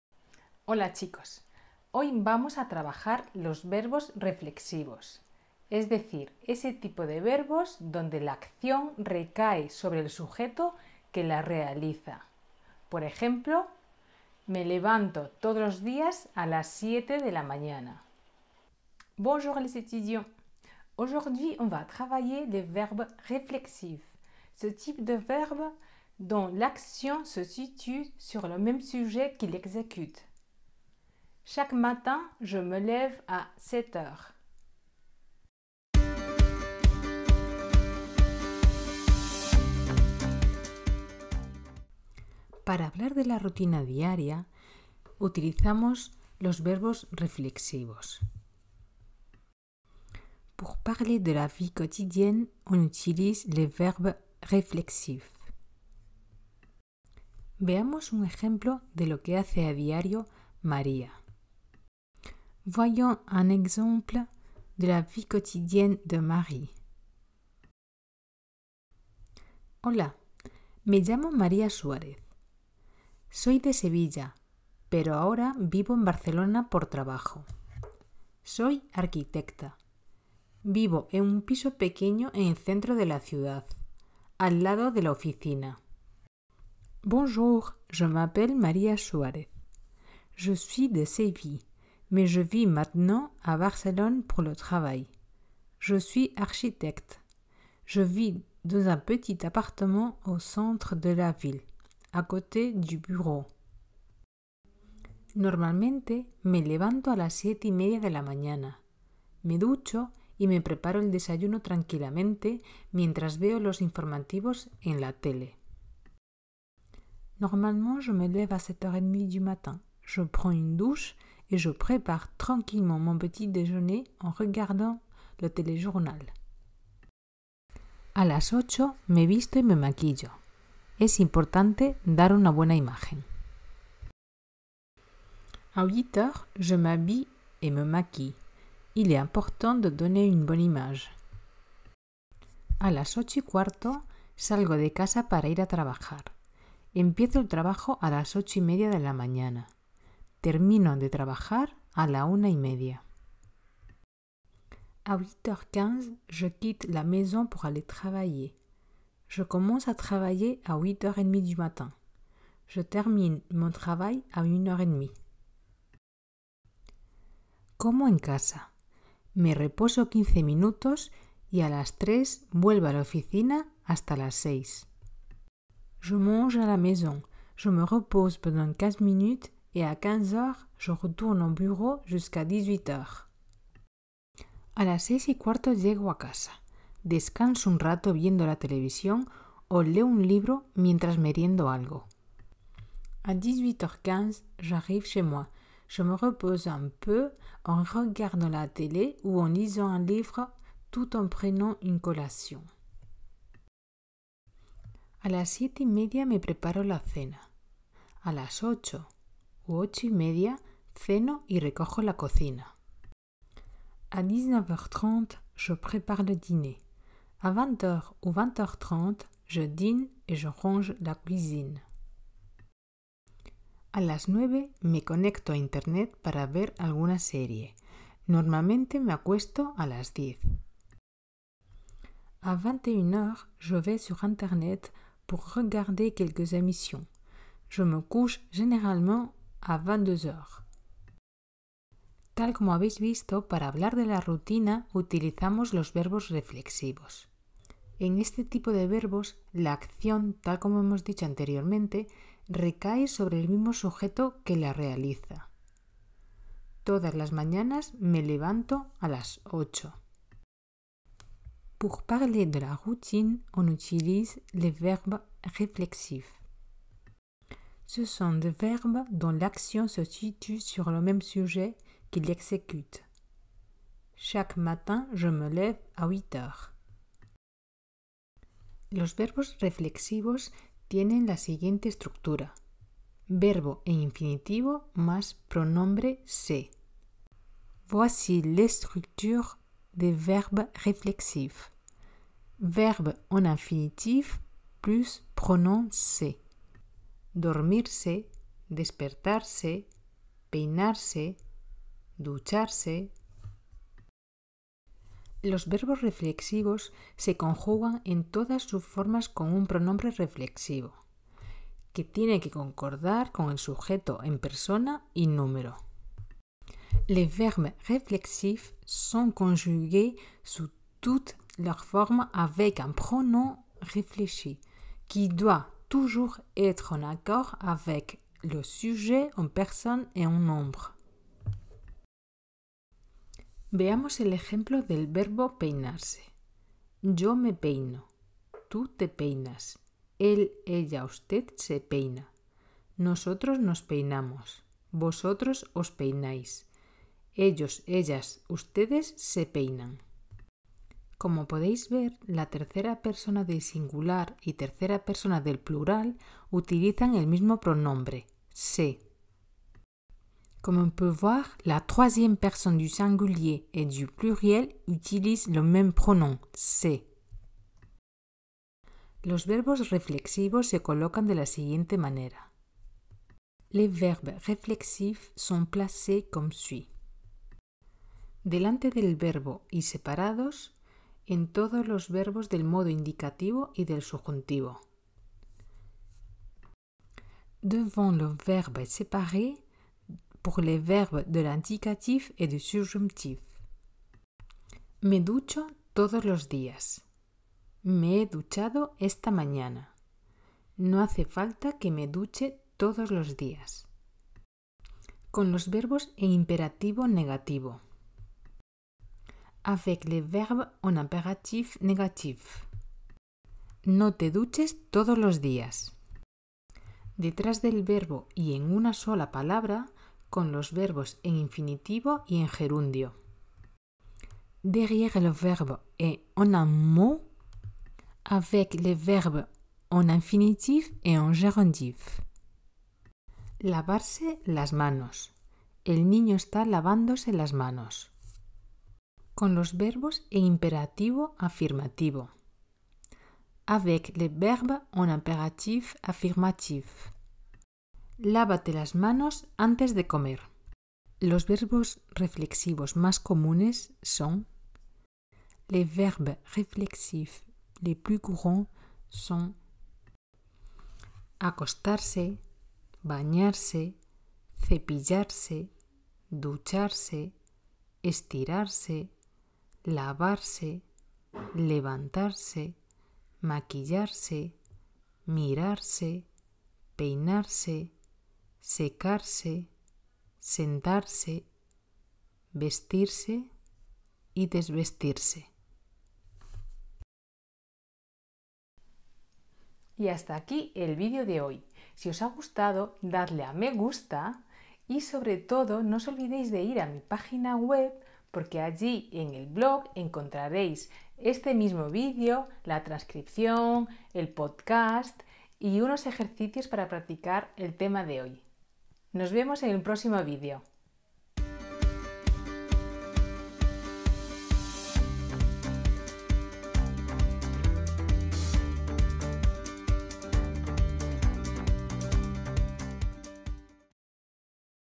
Les verbes pronominaux en espagnol
Los verbos reflexivos / Les verbes pronominaux
Para hablar de la rutina, utilizamos los verbos reflexivos. En este tipo de verbos, la acción, tal como hemos dicho anteriormente, recae sobre el mismo sujeto que la realiza: Todas las mañanas me levanto a las 8.

Pour parler de la routine, on utilise les verbes réflexifs. Ce sont des verbes dont l’action se situe sur le même sujet qui l’exécute. Chaque matin, je me lève à 8h00.

Los verbos reflexivos tienen la siguiente estructura: verbo en infinitivo más pronombre SE.
Voici la structure des verbes réflexifs: verbe en infinitif plus pronom SE: Dormirse despertarse, peinarse, ducharse.

Los verbos reflexivos se conjugan en todas sus formas con un pronombre reflexivo, que tiene que concordar con el sujeto en persona y número.

Les verbes réflexifs sont conjugués sous toutes leurs formes avec un pronom réfléchi qui doit toujours être en accord avec le sujet en personne et en nombre.

Veamos el ejemplo del verbo peinarse: Yo me peino, tú te peinas, él, ella, usted se peina, nosotros nos peinamos, vosotros os peináis, ellos, ellas, ustedes se peinan.
Como podéis ver la tercera persona del singular y tercera persona del plural utilizan el mismo pronombre “se”.
Comme on peut voir, la troisième personne du singulier et du pluriel, utilisent le même pronom “se”.

Los pronombre reflexivos se colocan de la siguiente manera. Les pronoms réfléchis sont placés comme suit:
Delante del verbo y separados en todos los verbos del modo indicativo y del subjuntivo.
Devant le verbe et séparés pour les verbes de l’indicatif et du subjonctif :
“Me ducho todos los días”.
“Me he duchado esta mañana”.
“No hace falta que me duche todos los días”.

Con los verbos imperativo negativo. Avec les verbes en impératif négatif:
“No te duches todos los días”.

Detrás del verbo y en una sola palabra con los verbos en infinitivo y en gerundio.
Derrière le verbe et en un mot avec les verbes en infinitif et en gérondif:
”Lavarse las manos”.
“El niño está lavándose las manos”.

Con los verbos imperativo afirmativo. Avec les verbes en impératif affirmatif. “Lávate las manos antes de comer”.

Los verbos reflexivos más comunes son. Les verbes réflexifs les plus courants sont:
Acostarse, bañarse, cepillarse, ducharse, estirarse, lavarse, levantarse, maquillarse, mirarse, peinarse, secarse, sentarse, vestirse y desvestirse.